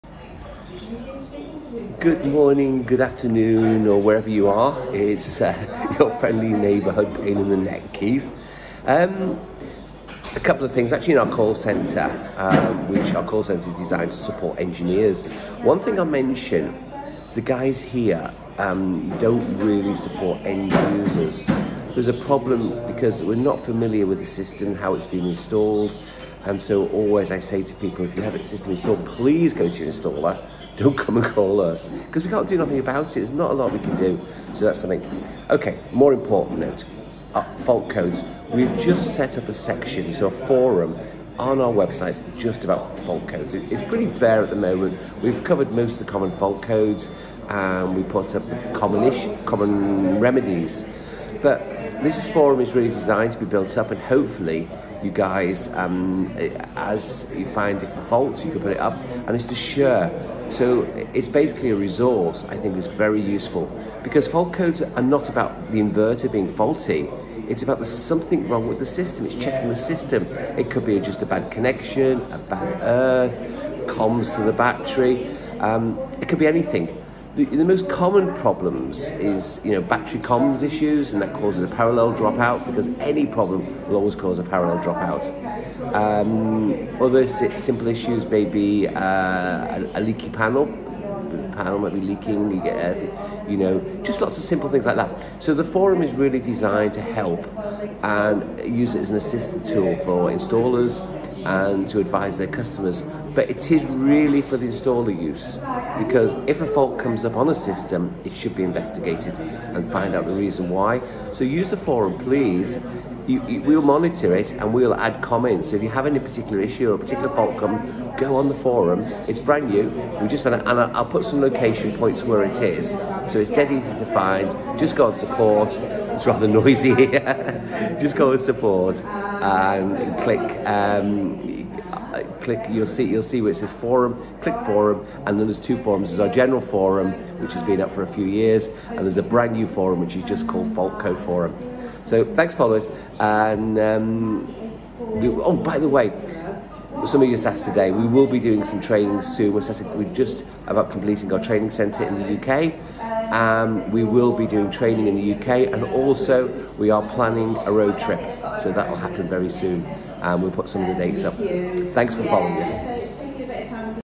Sunsynk fault code forum
Our Inverters go through very stringent tests at our factory. They are tested for everything, as well as long soak tests. When the inverter leaves the factory, it works perfectly. Therefore, any faults that appear are probably caused by installation issues.
To check fault codes, click on the ‘Fault Codes’ icon on the settings menu.